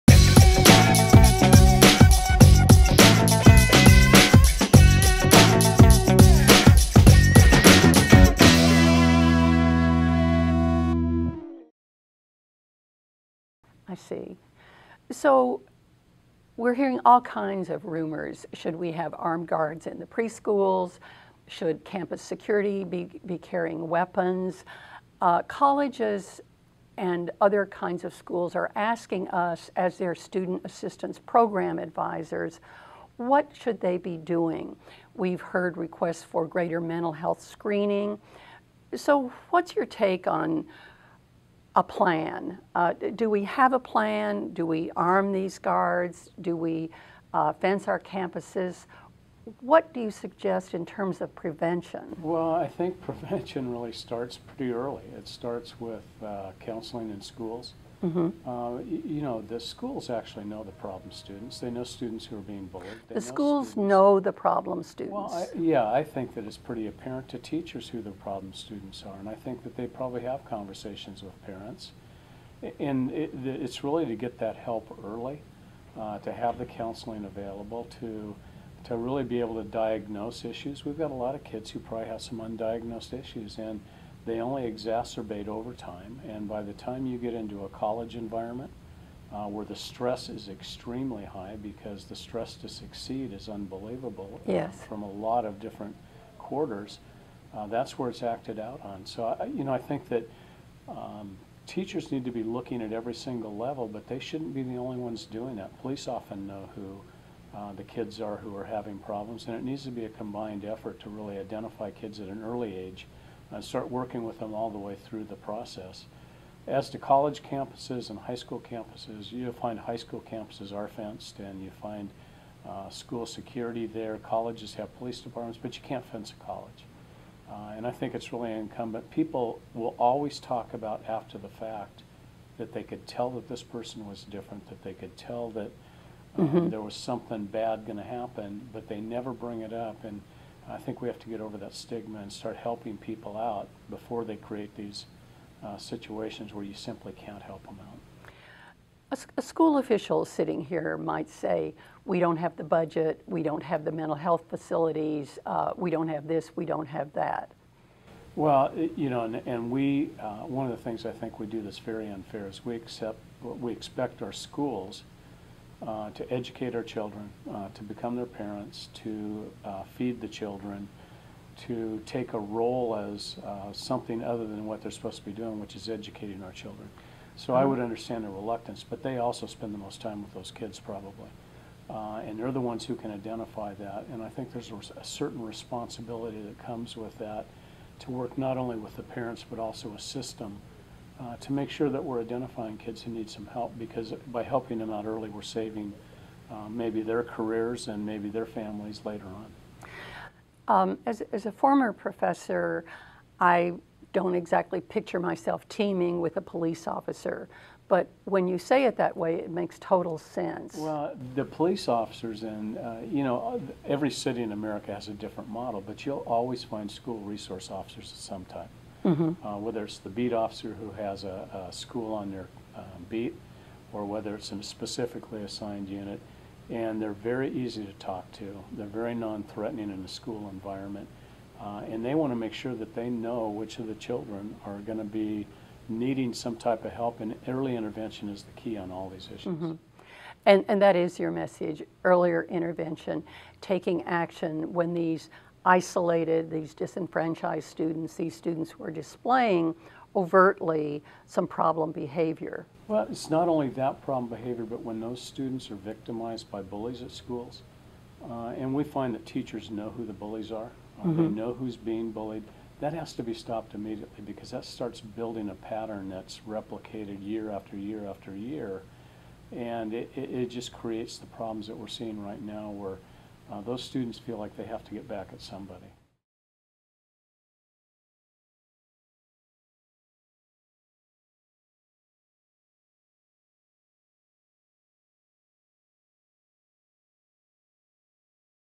Violence in Schools Featuring Jerry Sanders: Part 2
With the recent outbreak of school violence, ACI wanted to know more about what schools, teachers, parents, and student assistance programs can do to prevent violence and help save lives. CEO Dr. Ann Clark sits down with former Mayor of San Diego and Chief of Police Jerry Sanders to get an insider's view on practical tips for early recognition and prevention.

In Part 2, Jerry and Ann talk about how faculty, staff, families, police officers, and the community can partner together to bring about a solution to violence in schools.